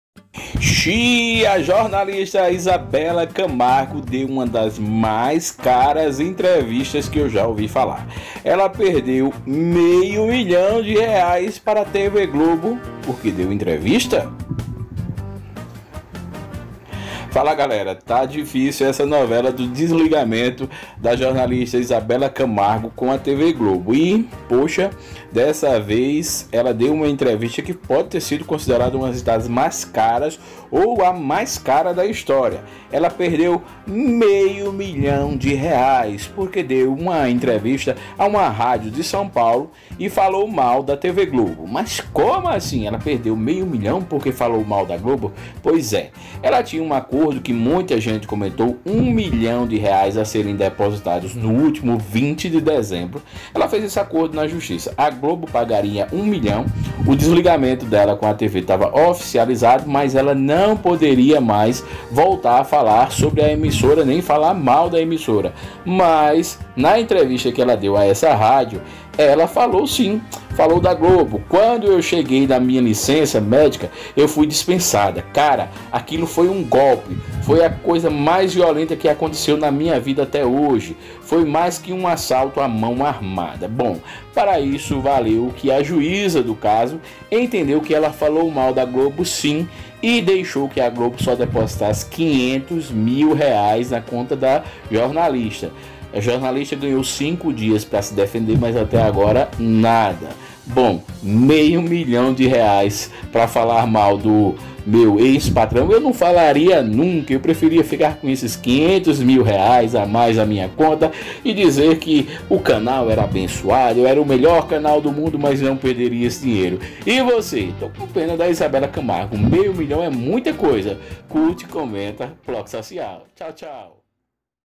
ENTREVISTA MAIS CARA DA HISTÓRIA FAZ IZABELLA CAMARGO PERDER MEIO MILHÃO PARA TV GLOBO
A saga de desligamento entre Izabella Camargo e a TV Globo continua. Dessa vez, o motivo que pode atrapalhar esse processo é a entrevista que jornalista concedeu ao programa “Pânico”, na Jovem Pan FM, no começo deste mês

De acordo com informações divulgadas na coluna de Ricardo Feltrin, no UOL, o acordo entre Izabella e a emissora determinava que ela não associasse mais o nome da Globo a sua doença, síndrome de Burnout, e não mais criticasse a empresa. Como indenização, a apresentadora receberia R$ 1 milhão.

Os advogados da Globo argumentam que a indenização não deve ser paga por completo, pois Izabella teria declarado que “Quando eu cheguei da minha licença médica e fui dispensada, cara, aquilo foi um golpe, foi a coisa mais violenta que aconteceu na minha vida até hoje. Foi mais do que assalto a mão armada”, durante a entrevista. O que configuraria quebra de acordo.

Dessa forma, o valor de R$ 1 milhão cairia pela metade, se transformando em R$ 500 mil. A juíza Raquel Marcos de Simões autorizou o depósito do valor menor, mas concedeu cinco dias para que Izabella apresente sua defesa em relação a entrevista vinculada no “Pânico”, dia 9 de dezembro.

Os comentários nesse vídeo não refletem o pensamento do PlocSocial ou do YouTube sendo inteiramente de responsabilidade do usuário/espectador.